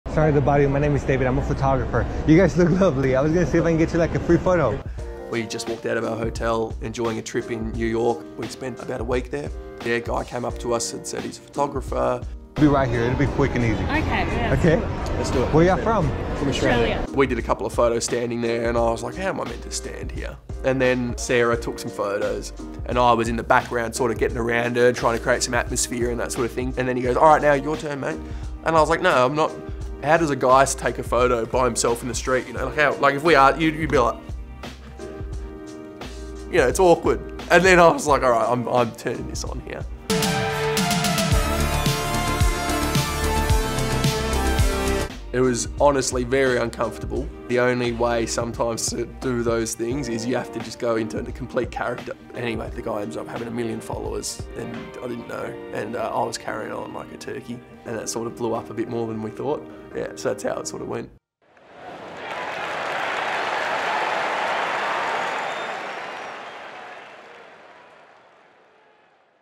Stoinis explains impromptu New York photoshoot | South Africa v Australia 2023
Marcus Stoinis and his partner Sarah were stopped on the street of New York City - and the Aussie allrounder explains what happened next